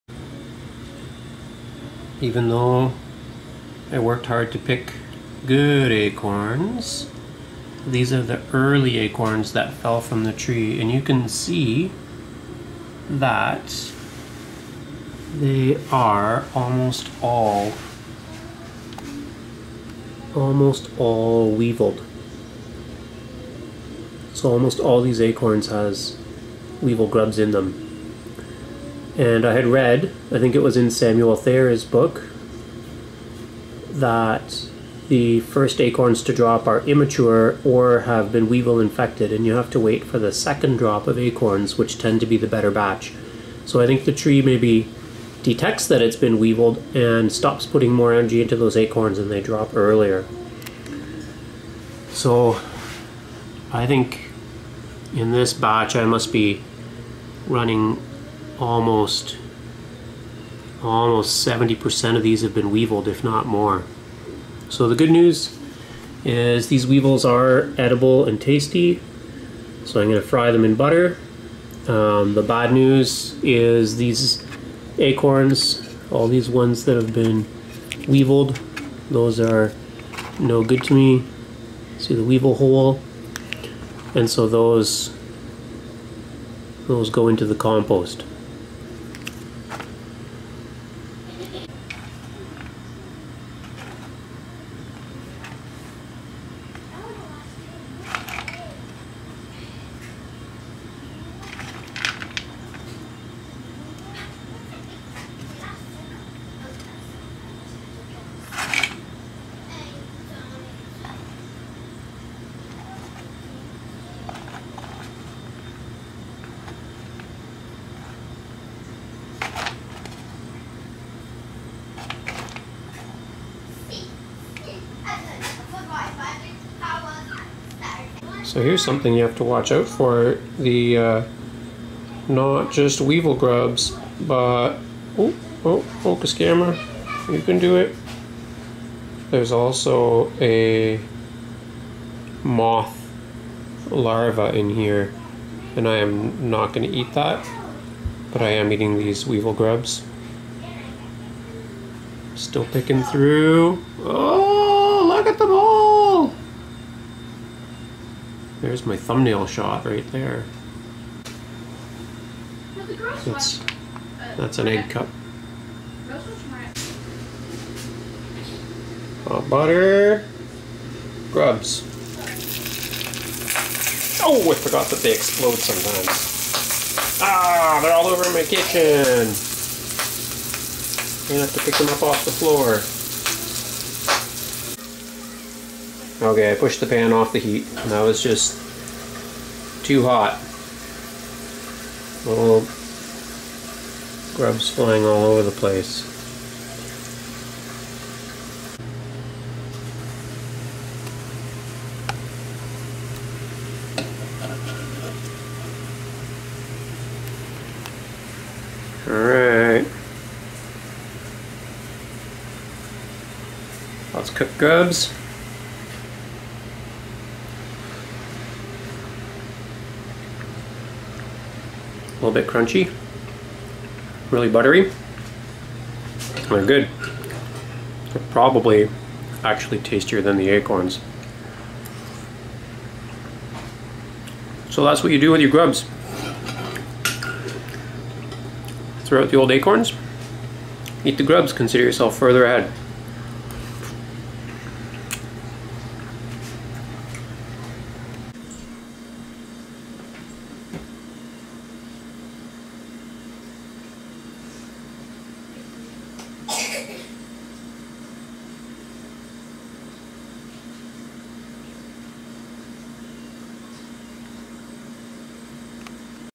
Catch and Cook Red Oak Acorn Weevils
Anyone who collects oak acorns knows that weevil infestation can be very high in certain years.  I no longer view this is as such a problem because I happily eat the weevil grubs.  Here I collect weevil grubs from drying acorns and cook them in butter,  Watch them pop like popcorn!